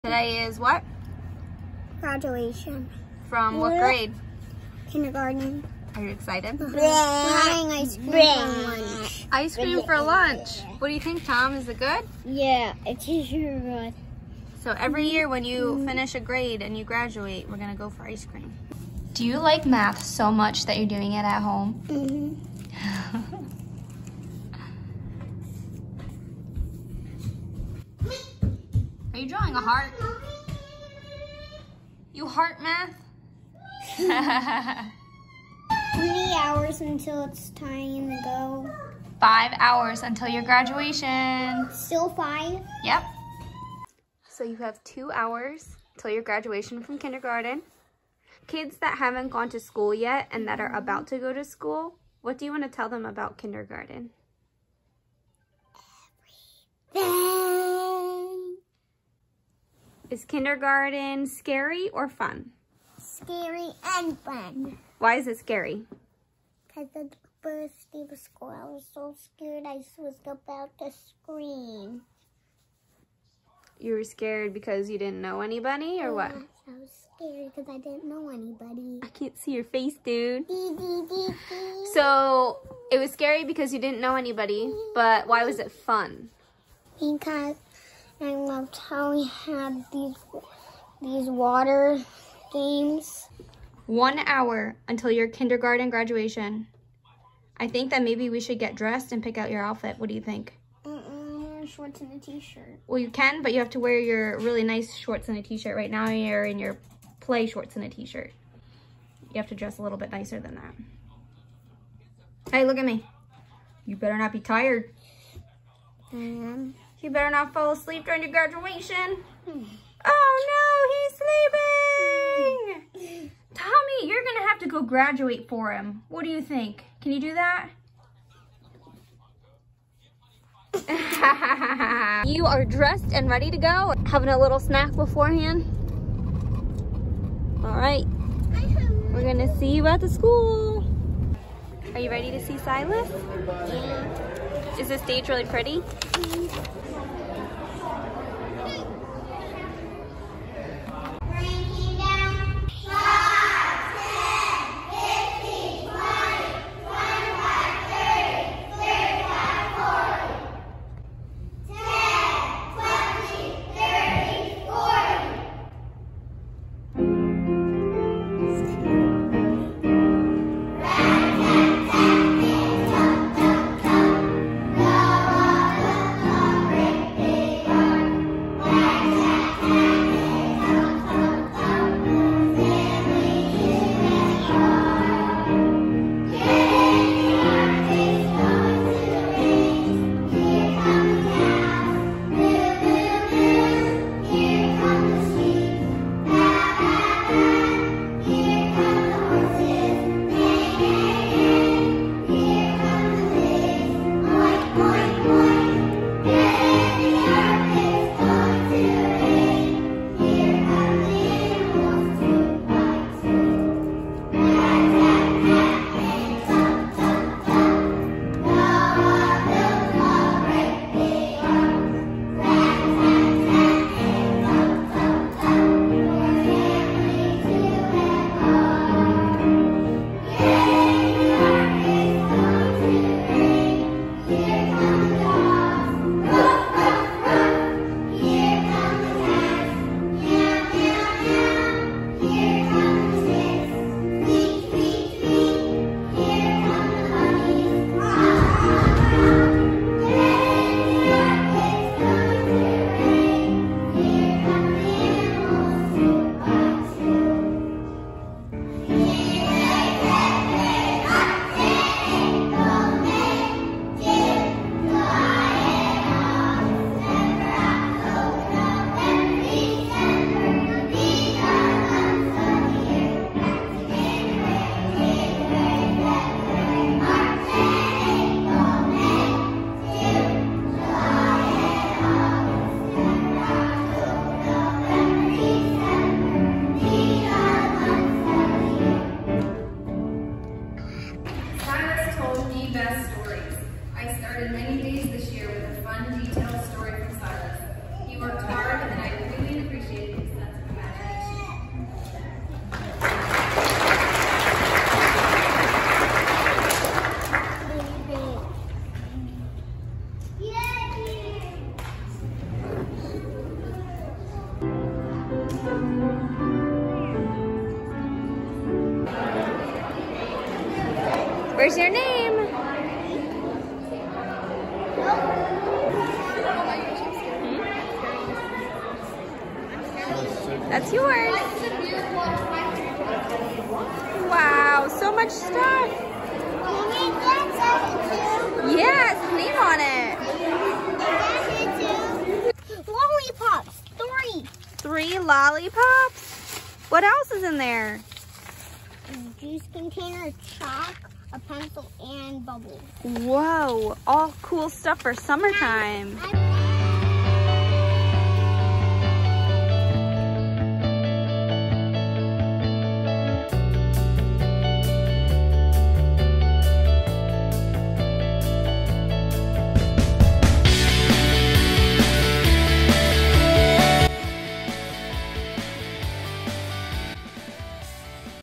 Kindergarten Graduation!
Silas graduates from Kindergarten! Watch as he counts down the hours until graduation. He shares his fears and favorite moments about kindergarten as well. Our hope is that any soon-to-be kindergartener that watches, sees how much fun it can be!